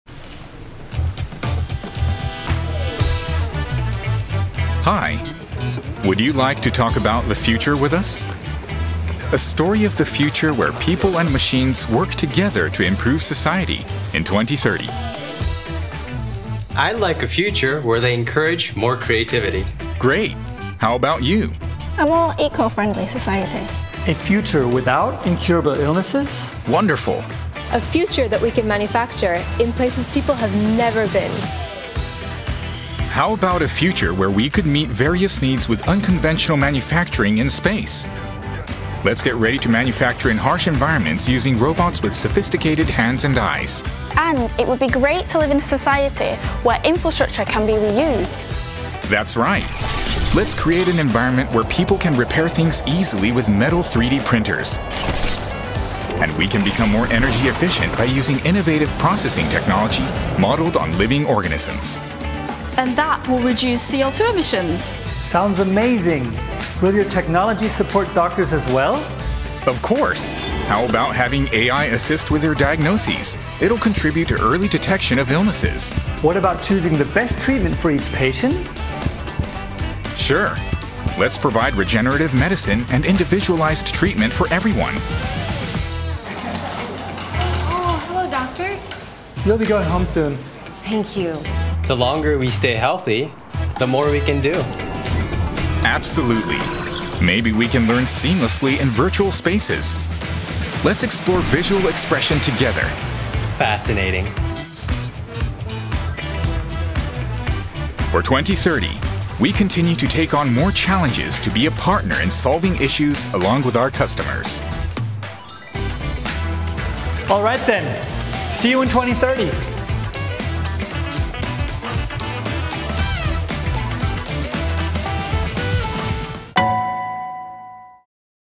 Vision 2030 | Nikon
To continue solving issues, in partnership with our customers, Nikon has been taking on new challenges towards the realization of "Vision 2030" – to be a key technology solutions company in a global society where humans and machines co-create seamlessly.
Here, you can learn more about our future envisions through the four areas of value proposition: factory, energy, healthcare, and life & entertainment.